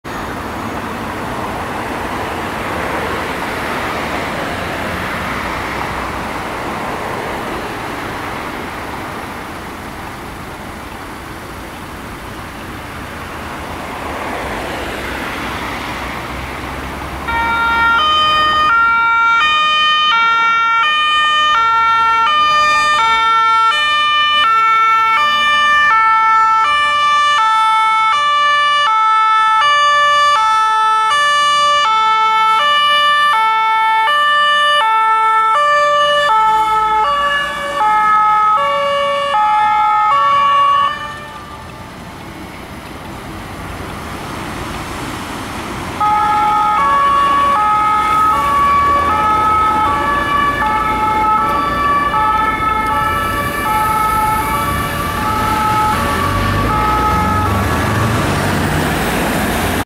ITW 57/87-2 DRK/ASB Darmstadt
Zusehen ist der Intensivtransportwagen aus Darmstadt auf Einsatzfahrt in Mainz. Einsatzgrund war vermutlich eine Verlegung.